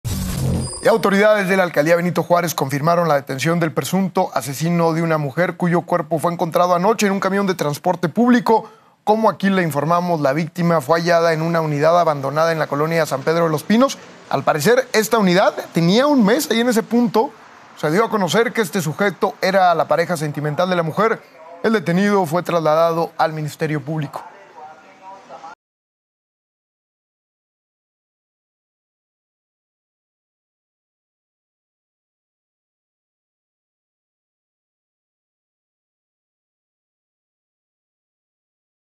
Detienen a presunto asesino de mujer hallada en camión; Era su pareja - Las Noticias
Las autoridades de la alcaldía Benito Juárez confirmaron la detención de un sujeto que presuntamente asesinó a una mujer en un camión abandonado en la colonia San Pedro de Los Pinos.